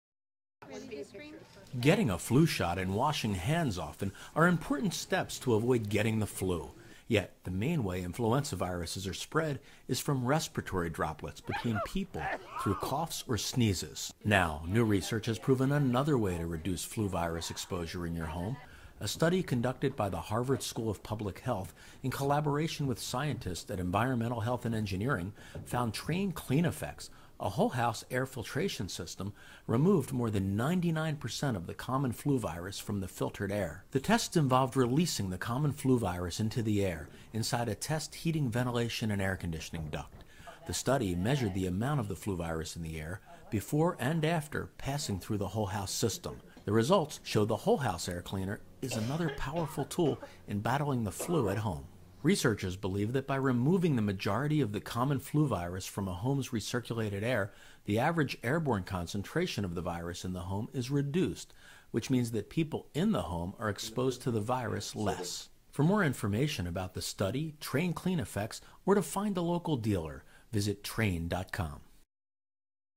Trane CleanEffects Removes Flu Virus From Filtered Air
If you want to protect yourself from catching the flu in your own home, Trane might be able to help.  First-of-its-kind research on removing flu virus conducted by researchers at the Harvard School of Public Health, in collaboration with scientists at Environmental Health and Engineering Inc., (EH&E) shows that Trane CleanEffects, a whole-house air filtration system from Trane Air Conditioning, removes more than 99 percent of the common flu virus, or influenza A virus from the filtered air.